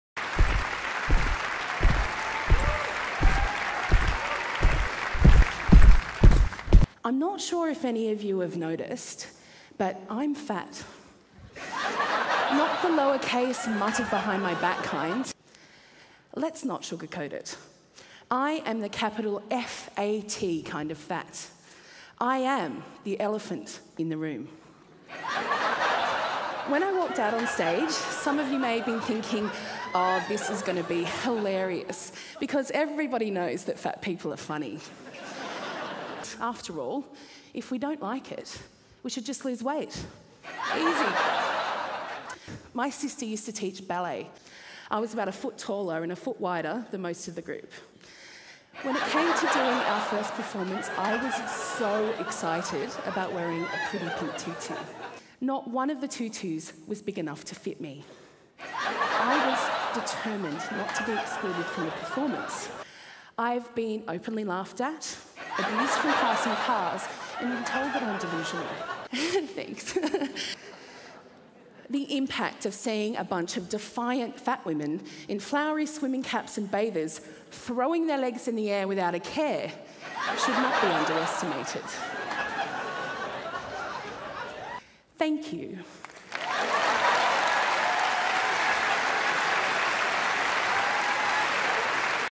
Kellie Jean Drinkwater the Hog Does Fat Stand Up Routine
After gaining another 40 kg, Kellie Jean Drinkwater the Hog performs her comedy skit.  When it comes to comedy, we all know that Kellie is a heavyweight, and that stage she's standing on knows that too!